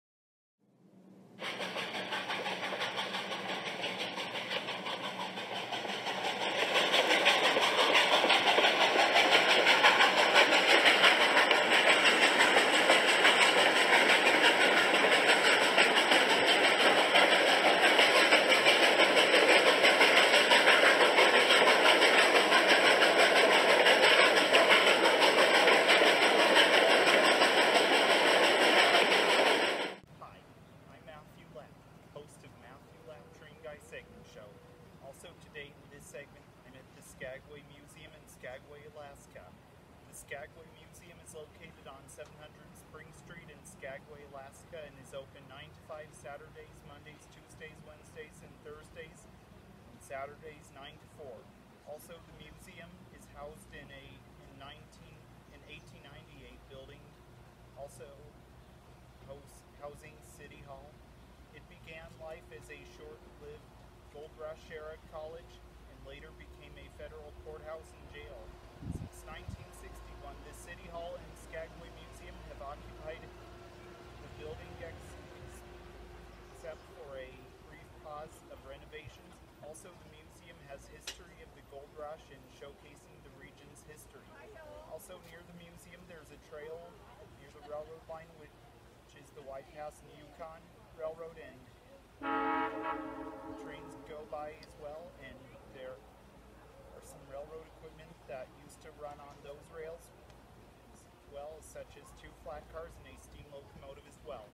_Skagway Museum - Skagway, AK_ Episode 205 (Beginning)
Skagway Museum
Historic Museum in Skagway near the Railroad and some Railroad equipment adjacent to the museum
_MLTG_